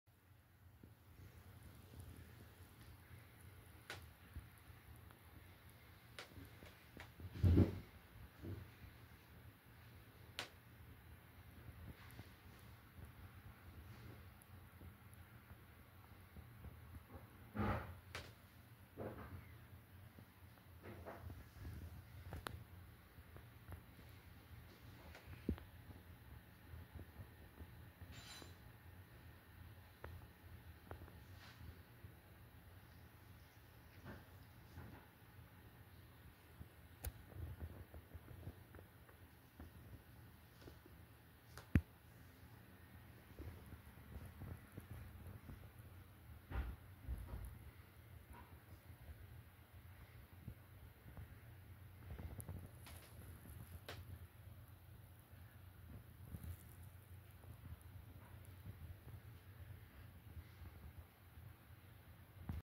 Happy Cat Rubs Head on Bed
A cat rubbing its head against a bed is often associated with marking territory and expressing affection. As the cat approaches the bed, it may nuzzle its head against the soft surface, using its scent glands around its face to leave its mark. This action can be accompanied by purring or kneading, indicating comfort and contentment.